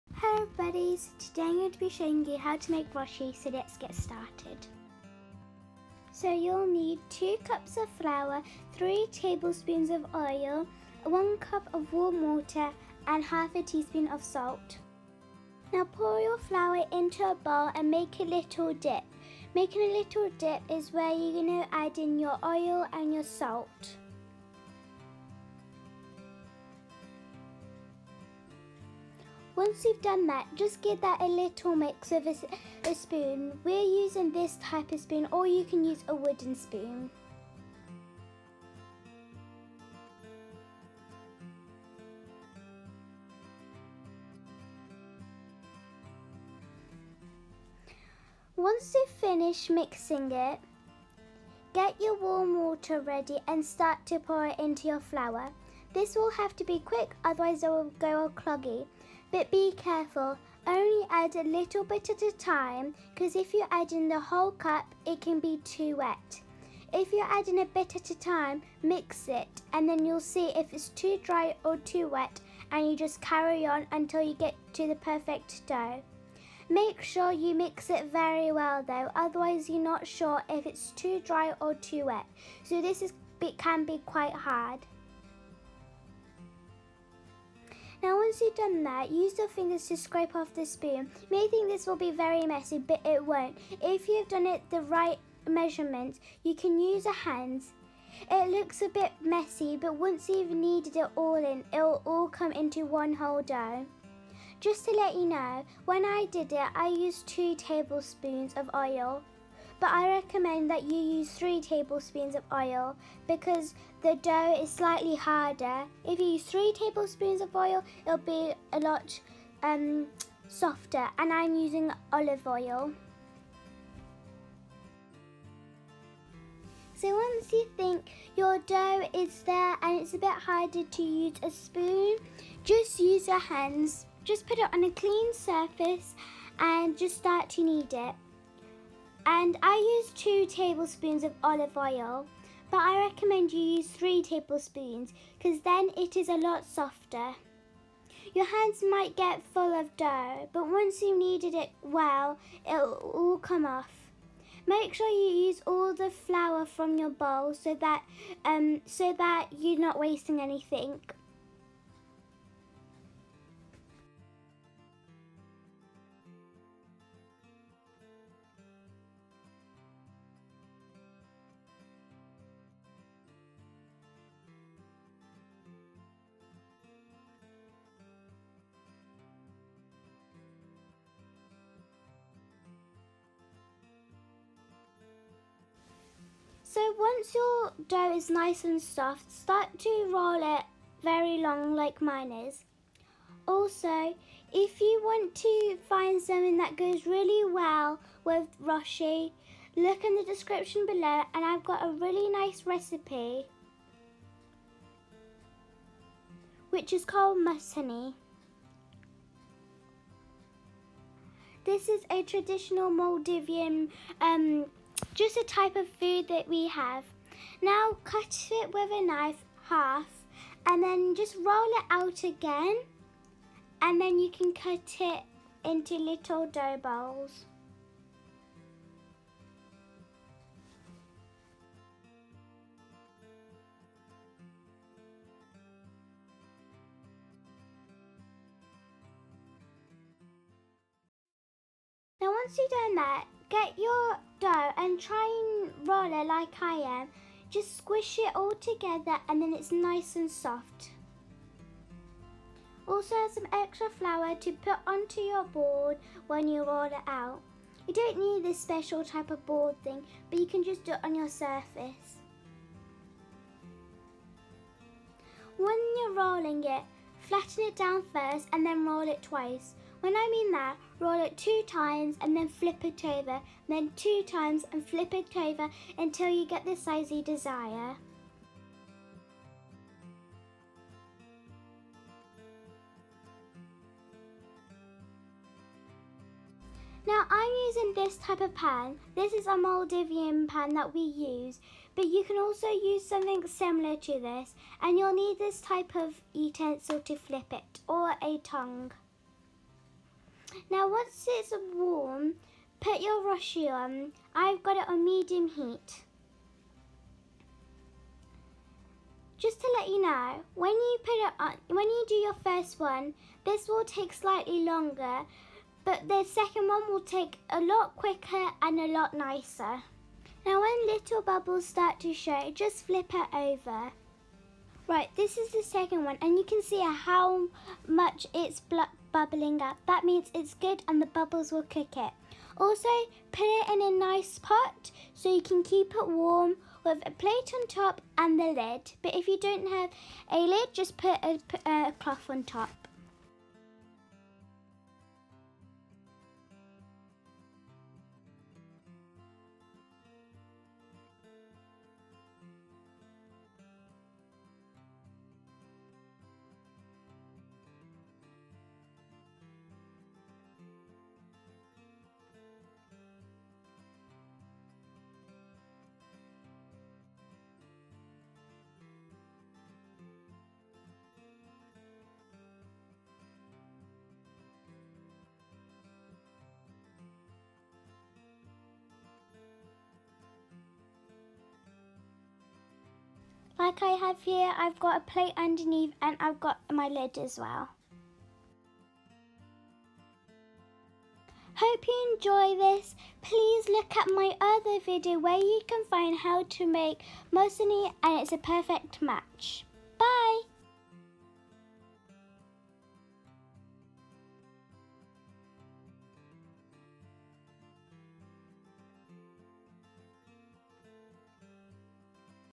How to make Roshi - Roti - Flatbread - Cooking with Zed
Hello everyone! Sorry this video took a long time to upload, but here it is. This is a traditional type of bread to have with Mashuni (Here is the link for it)
And here are the ingredients to make roshi!

2 cups of flour
3 tablespoons of olive oil (or any type of cooking oil)
1 cup of warm water

Any requested videos please type them in the comment section and i will try and make them and i will see you in my next video bye